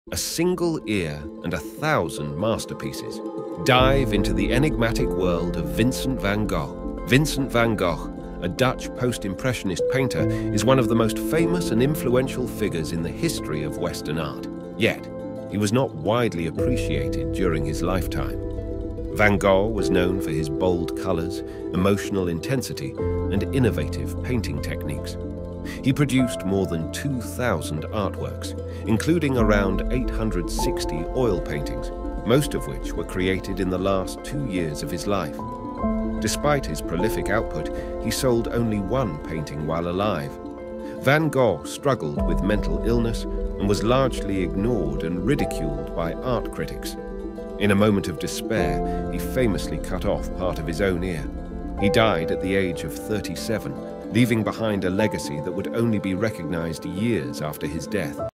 The Tortured Genius
Description:
Step into the vibrant and tumultuous world of Vincent van Gogh, a Dutch Post-Impressionist painter whose innovative techniques and emotional intensity have made him one of the most influential figures in Western art. Discover the man behind over 2,000 artworks, who, despite facing ridicule and mental illness, painted masterpieces that were only fully appreciated after his untimely and tragic death, leaving a legacy of bold colors and expressive brushwork that continues to captivate art enthusiasts worldwide.

Generate stunning AI art with getimg ai!